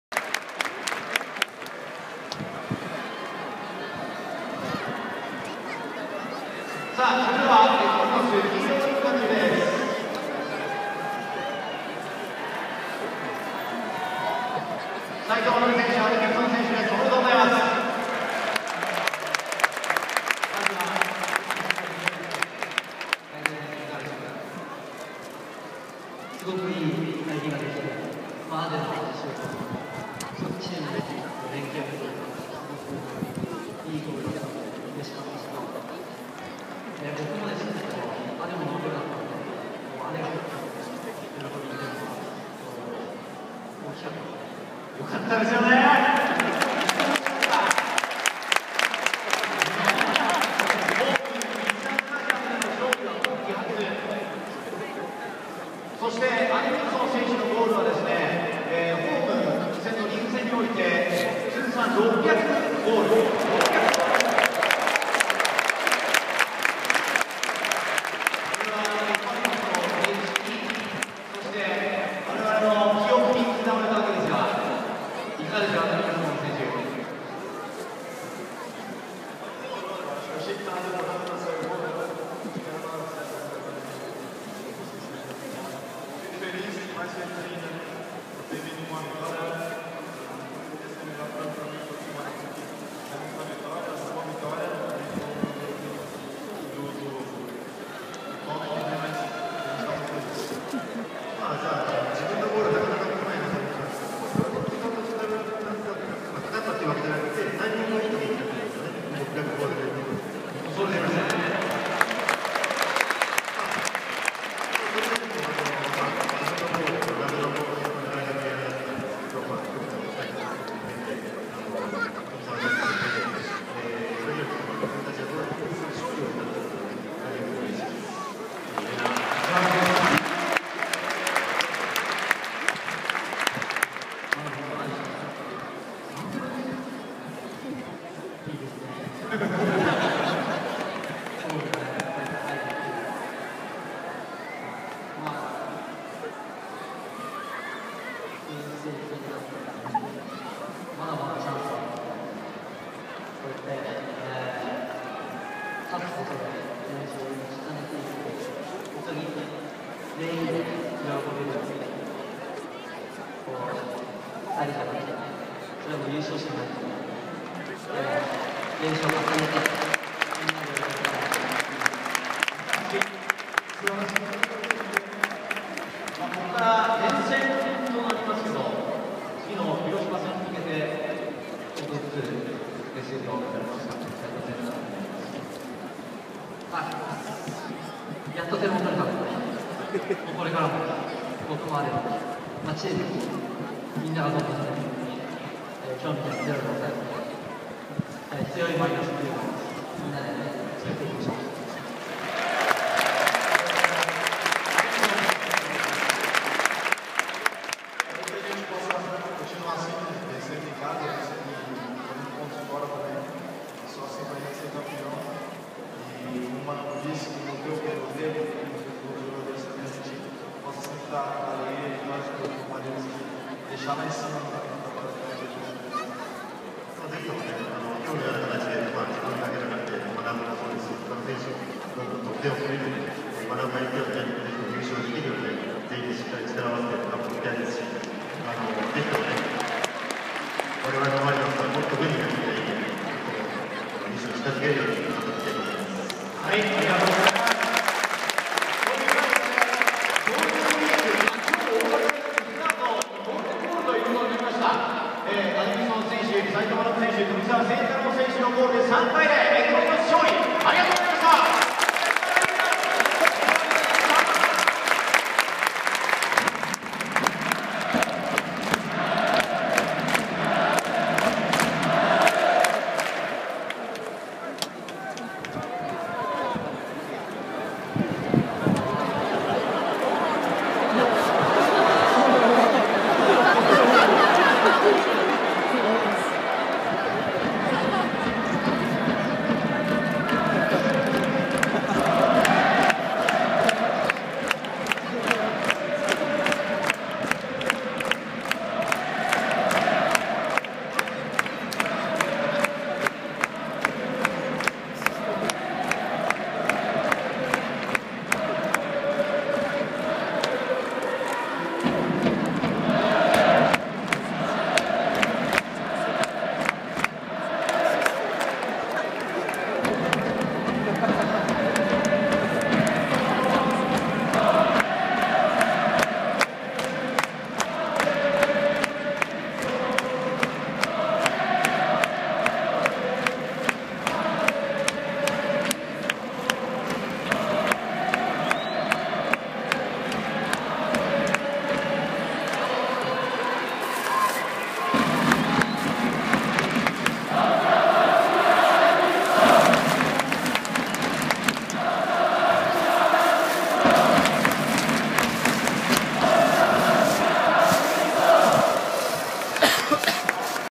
2015 J1 1st 第7節 横浜F・マリノス v 湘南ベルマーレ 齋藤学&アデミウソンのヒーローインタビュー
2015年04月25日(土) 15時キックオフの横浜F・マリノスvs湘南ベルマーレ 試合後、待望の来日初ゴールを決めたアデミウソンとシーズン初ゴールを決めた齋藤学へのヒーローインタビューの様子です。